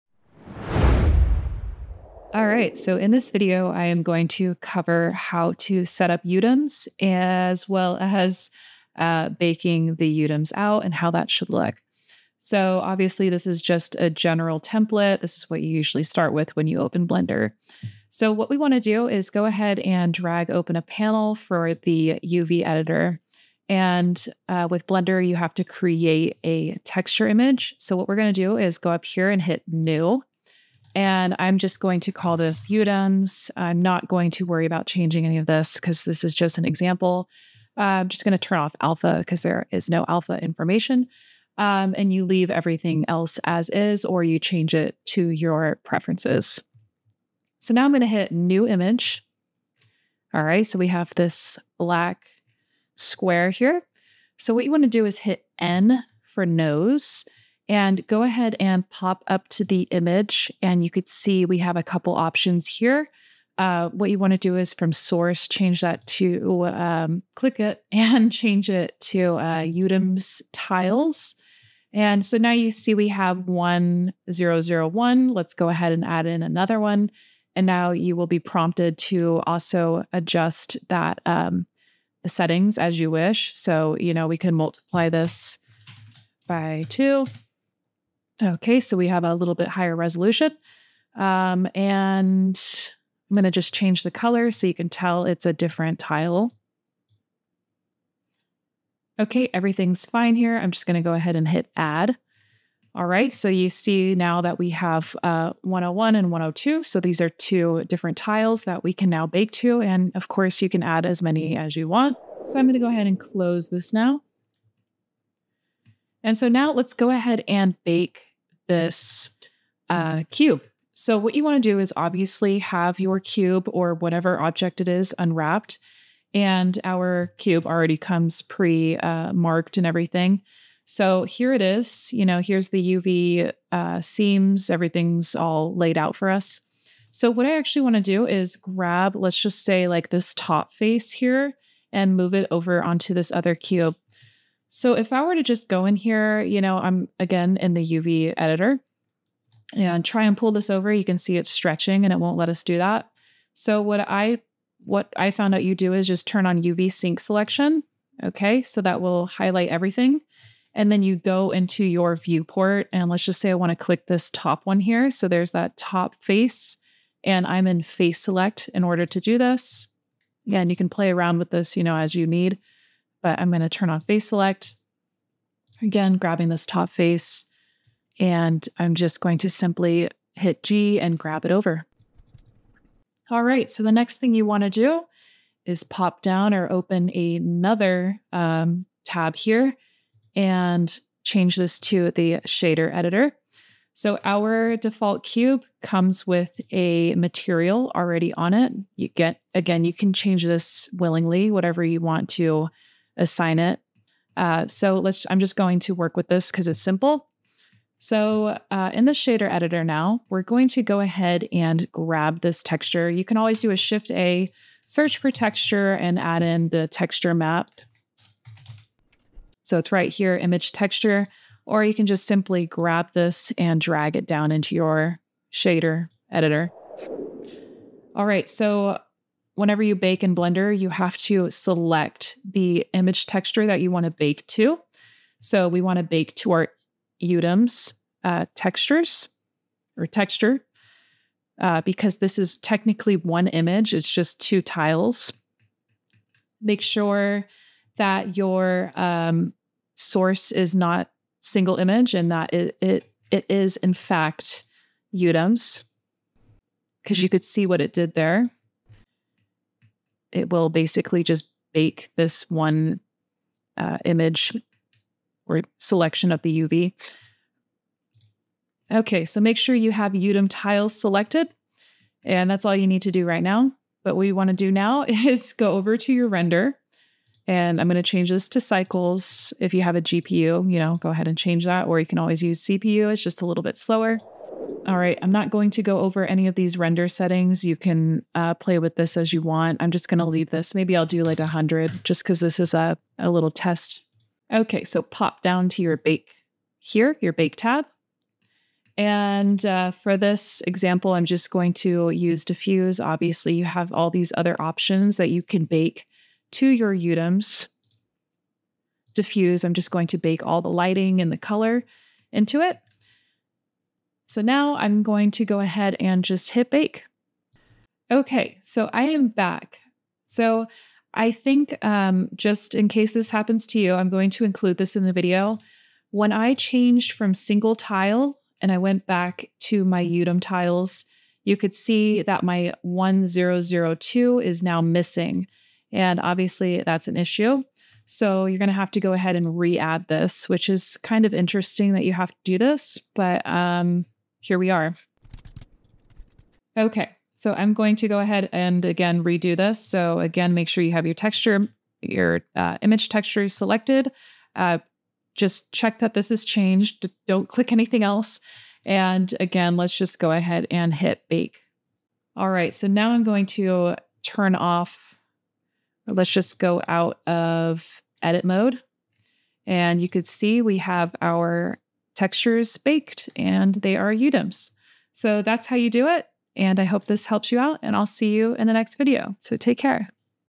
How to Set-Up and Bake UDIMS in Blender
I'll go ahead and show you how to set-up and bake out your UDIM texture map.

It's actually super simple, so here ya go! Hope it helps you out :)